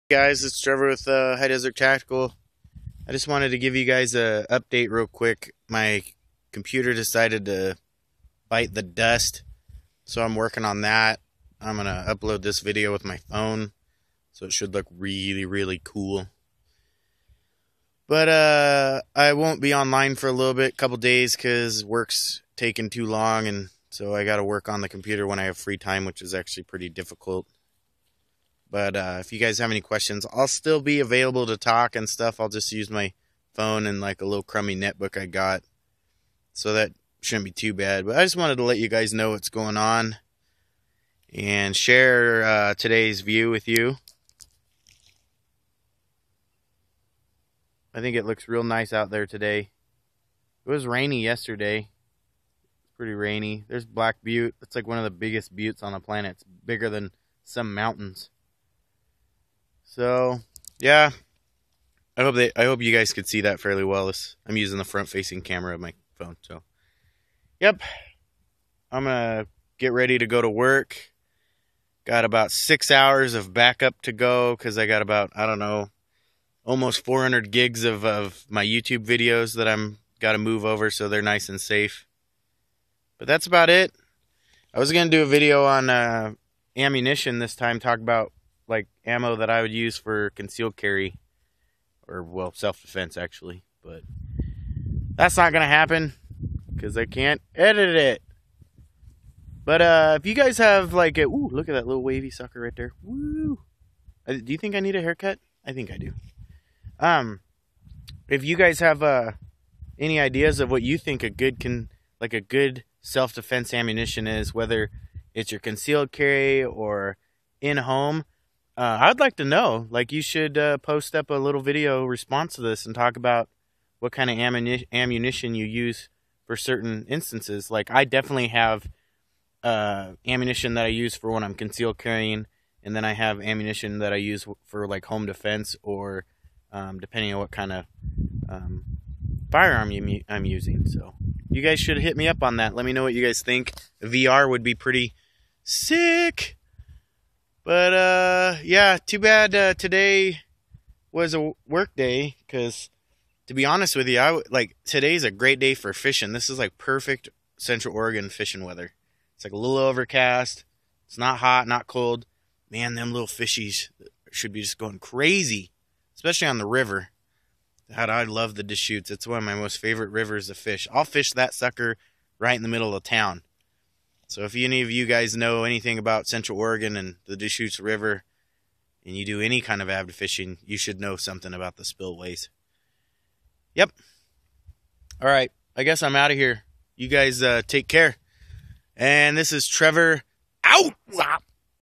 What is your self defense ammunition?
Just wanted to let you guys know my pc is out of commission. I did have a question what kind of self defense ammo do you use? What type of situations and why? Post a VR tell me what you think!